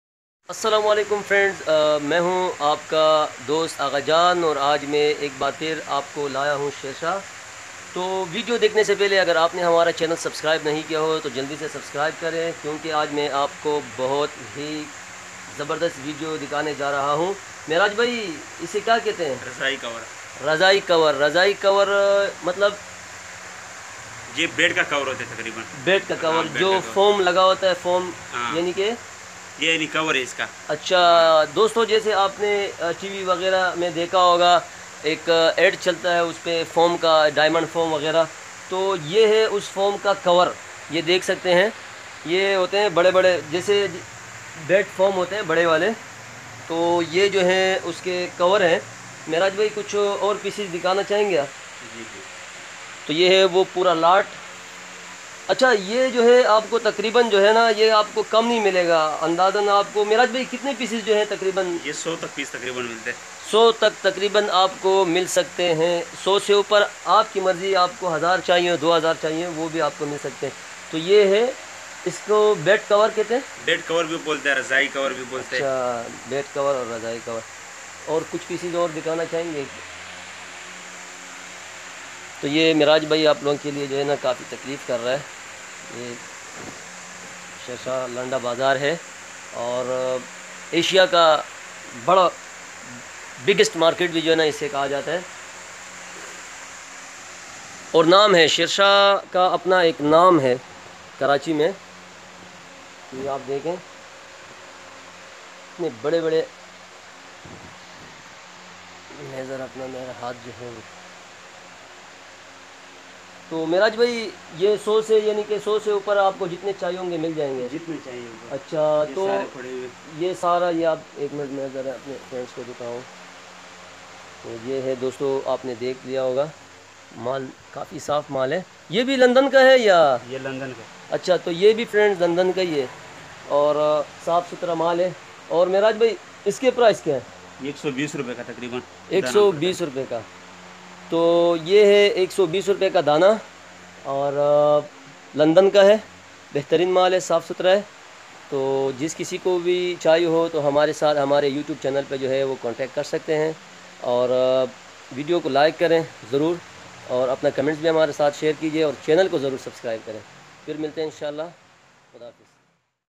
Imported Bed Cover | Shershah Landa Godam Cheap Rates | Agha Jan
Bed Cover | Bed Sheet | Imported Bed Cover  | Cotton Bed sheets | Agha Jan

Assalam O Alaikum! My name is Agha jan I am from Karachi.

Today, I will give you details about Ladies Scarf Imported A Quality cheap rate Shirts Imported from UK etc. which is available at Shershah Godam Karachi,, You can start a business from a small investment.